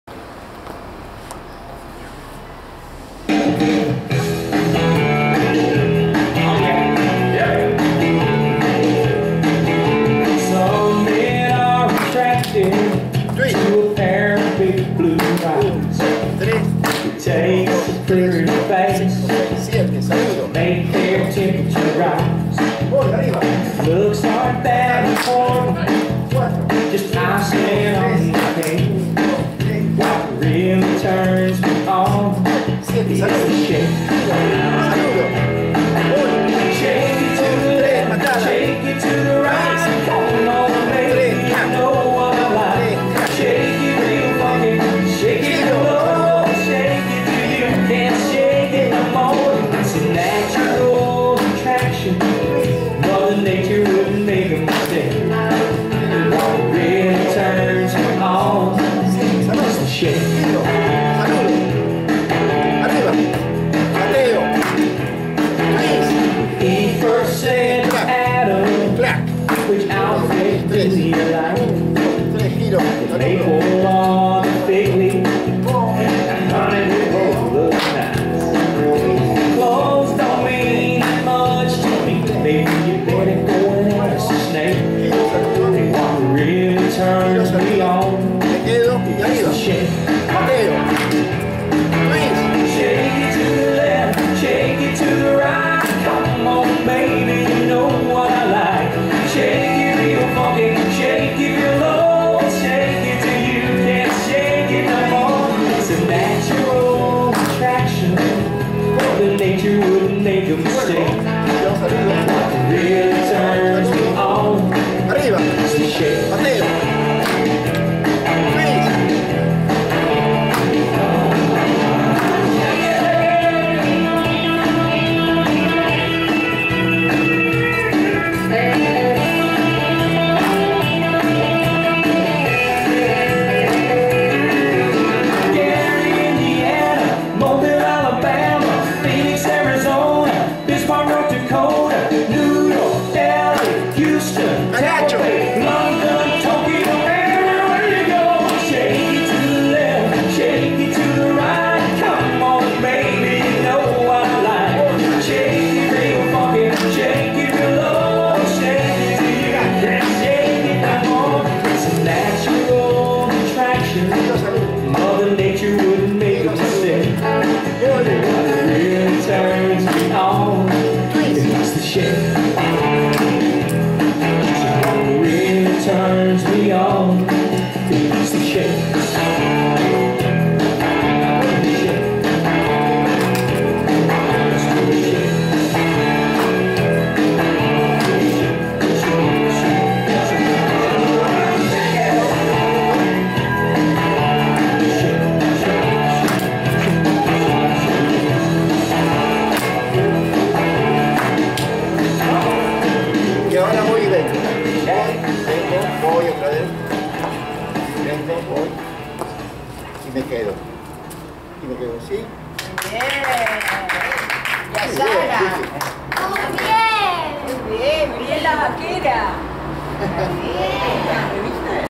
04 The shake con Vicky & Mary 2020 03 20 195538
Bailando en la terraza del edificio.
Street Line Dance Country